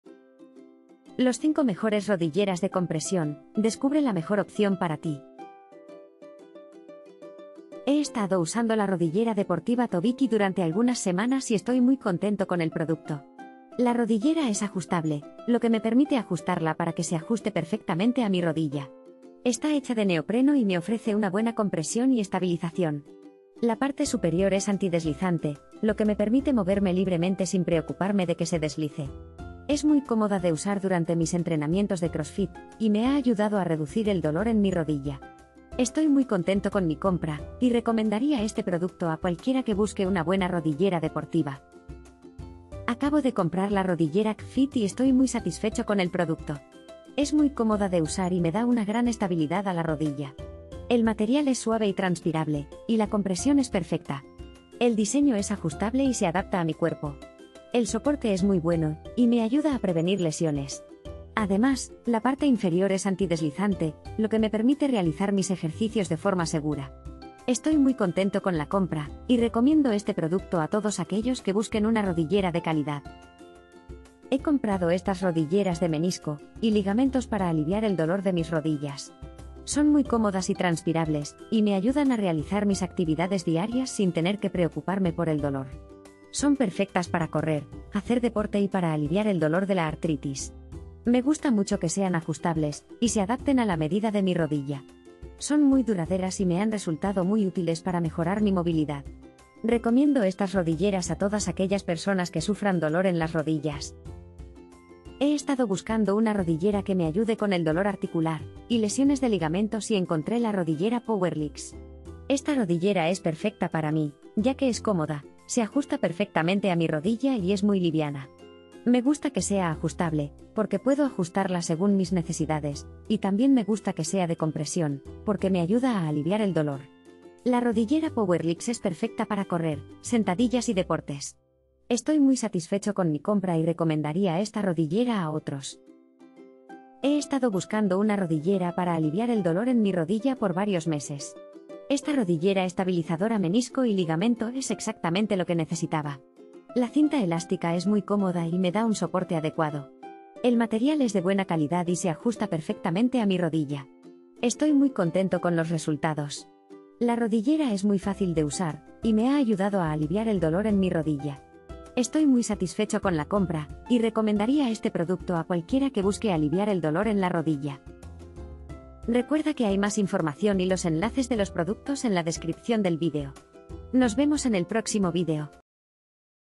Rodillera de compresion: Los 5 Mejores Rodilleras de Compresión: ¡Descubre la Mejor Opción para Ti!
✋  Aquí están los 5 mejores rodilleras de compresión: ¡descubre la mejor opción para ti! disponibles en el mercado.

🔽🔽🔽 ENLACES DE COMPRA RODILLERA DE COMPRESION 🔽🔽🔽

1) TOVIKI Rodillera Menisco Ligamento Deportiva Ajustable Protector de Rótula Abierta Rotuliana Antideslizante Neopreno Compresión Estabilizadora para Correr Crossfit

2) CPFITT Rodillera Menisco, Ligamento, Protector de Rótula, Ajustable, Estabilizadora, Gym, Crossfit, Correr, Compresión, Rotuliana, Antideslizante, Deportiva, Talla única, Hombres y Mujeres

3) Rodilleras Menisco y Ligamentos, 2 Piezas Rodilleras Transpirable para Hombre y Mujer, Rodillera Deportiva Compresion para Artritis, Alivio del Dolor, Desgarro de Menisco, Correr, Deportes XL:48-55CM

4) POWERLIX rodillera para mujeres y hombres, rodillera de compresión para artritis, dolor articular, lesión de ligamentos, desgarro de menisco, tendinitis, correr, sentadillas, deportes, Negro, L

5) Rodillera Estabilizadora Menisco y Ligamento y Compresión Gracias a Su Cinta Elástica que Estabiliza y Alivia la Rodilla. Rodillera Deportiva Ajustable para Hombre y Mujer. Incluye 2 Vendas Elásticas

00:00 - Intro
00:06 - 1) TOVIKI Rodillera Menisco Ligamento Deportiva Ajustable...
00:41 - 2) CPFITT Rodillera Menisco, Ligamento, Protector de Rótula,...
01:13 - 3) Rodilleras Menisco y Ligamentos, 2 Piezas Rodilleras...
01:43 - 4) POWERLIX rodillera para mujeres y hombres, rodillera de...
02:15 - 5) Rodillera Estabilizadora Menisco y Ligamento y Compresión...
02:47 - Outro